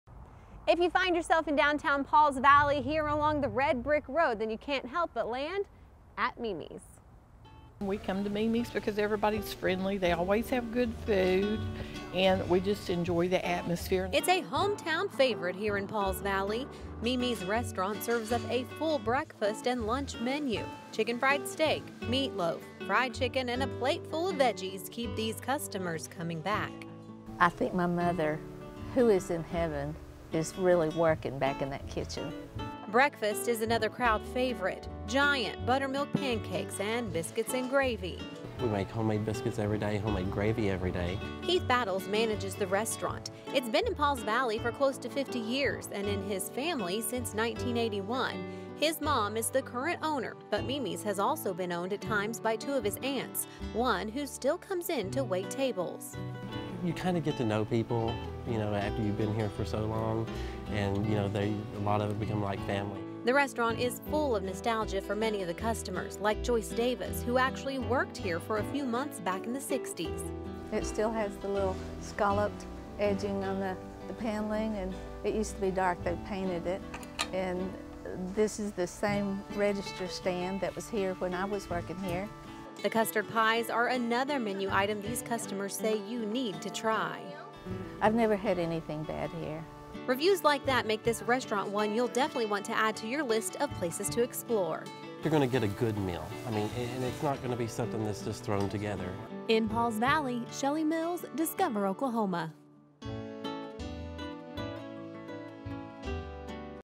Mimi's Restaurant
Mimi’s Restaurant in Pauls Valley is a hometown favorite! They serve up a full breakfast and lunch menu, from chicken fried steak, meatloaf, fried chicken with veggies! But, breakfast is big here! From giant buttermilk pancakes, homemade biscuits with gravy, what more could one ask for? The custard pies are another menu item many customers say you need to try! Come to Mimi’s Restaurant and find out why customers say all the food takes like home.